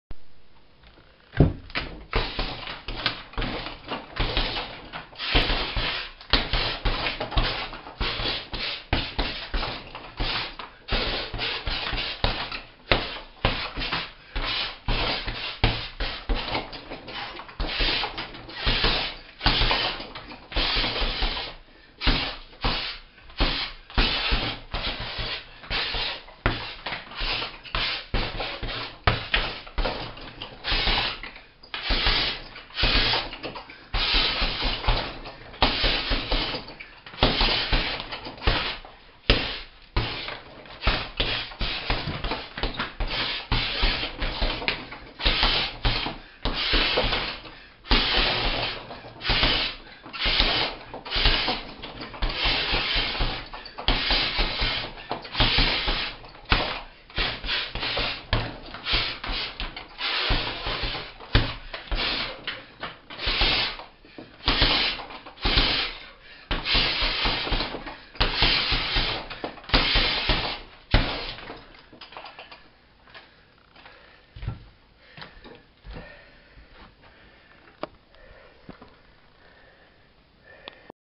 1 Minute und 9 Sekunden Boxen am Sandsack (Freie Kombis)
Am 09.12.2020 hab ich mich nach meinem Kraftsporttraining noch 1 Minute und 9 Sekunden (69 Sekunden) lang an meinem Sandsack in Bilfingen im Hobbyraum meiner Eltern ausgepowert (Freie Kombis) (Indoor) 👍💪😁👊🥊